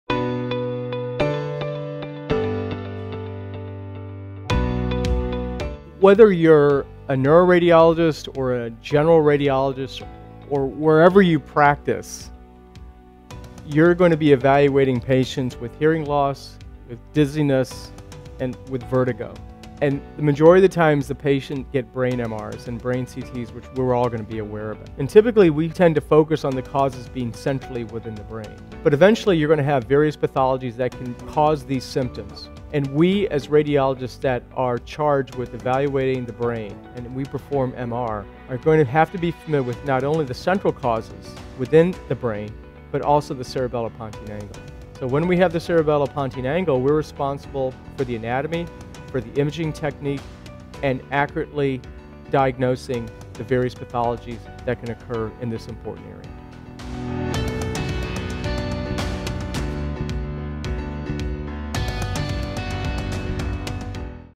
MRI Mastery Series -- CP Angle Masses (Trailer)
Whether you're a neuroradiologist or a general radiologist you're going to be evaluating patients with hearing loss, with dizziness and with vertigo. In the majority of cases, patients will have brain MRI's and CT's performed.  Typically we tend to focus on the causes being centrally within the brain, but eventually you're going to have various pathologies that can cause these symptoms. As radiologists we are charged with evaluating the brain and we perform MRI and we are going to have to be fluent with not only the central causes within the brain, but also the cerebellopontine angle. When we evaluate the cerebellopontine angle, we're responsible for the anatomy, for the imaging technique and accurately diagnosing the various pathologies that can occur in this important area.